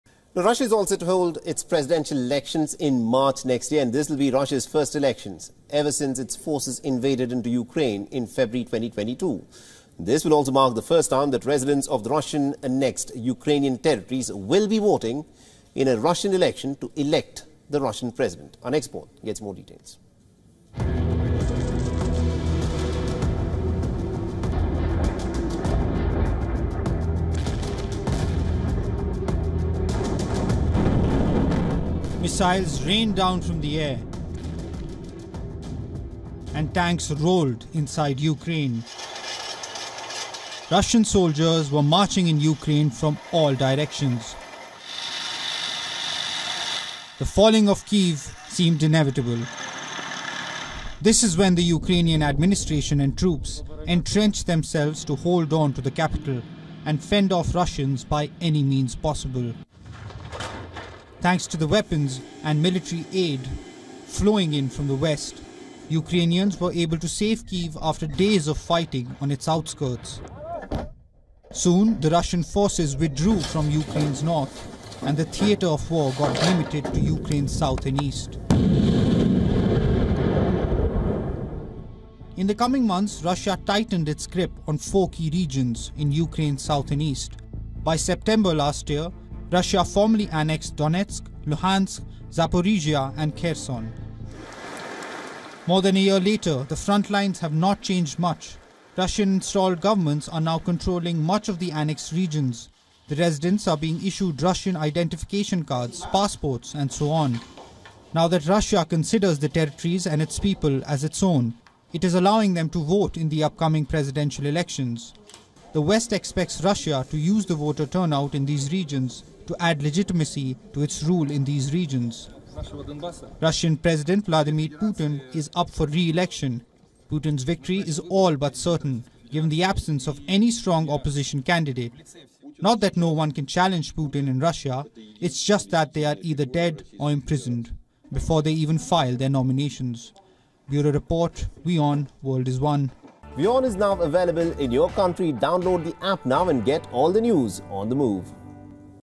Russia to hold presidential election in four annexed Ukrainian regions | WION Fineprint
Russia's presidential election next March will include voting in four regions of Ukraine that Russia claimed as its own territory last year, Interfax news agency quoted the central election commission as saying on Monday.